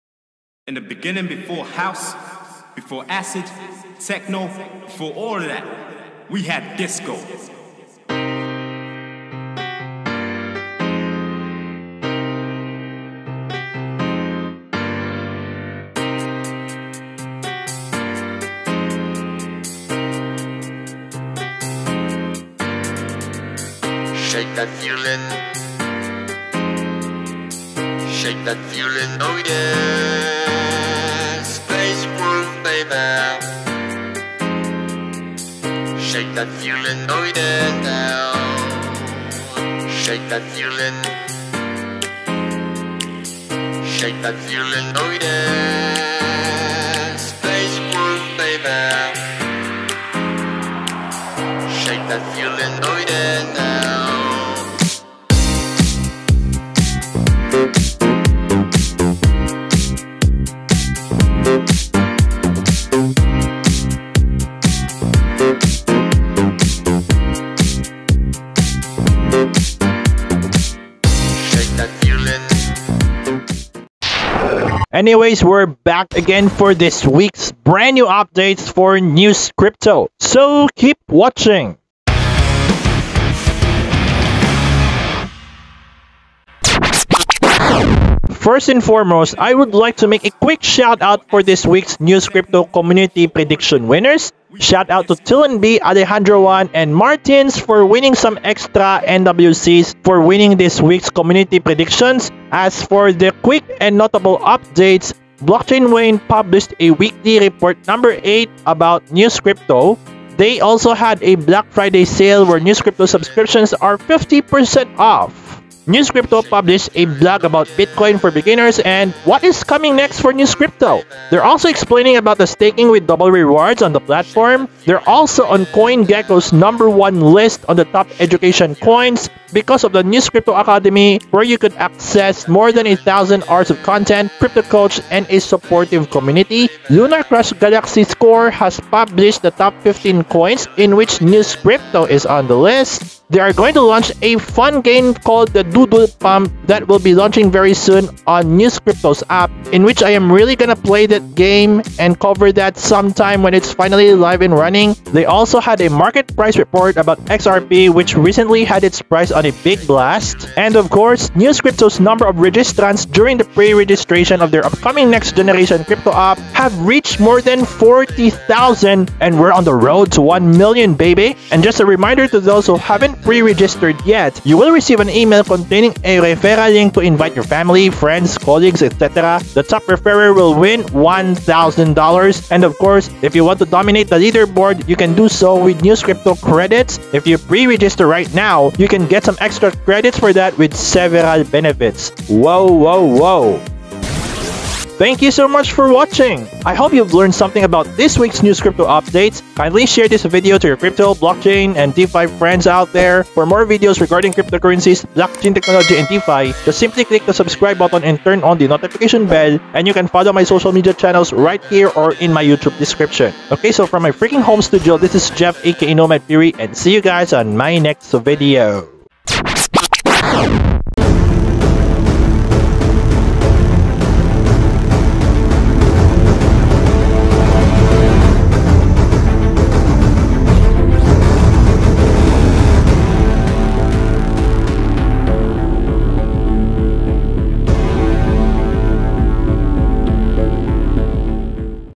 Newscrypto November 2020 Update #5
Hey there peeps! It's me once again for another weekly Newscrypto update. We are just a day away from our favorite month of the year, so this week's Newscrypto updates like it's next-generation app is pretty much exciting. If you wanna learn more about these updates, I suggest that you should keep watching!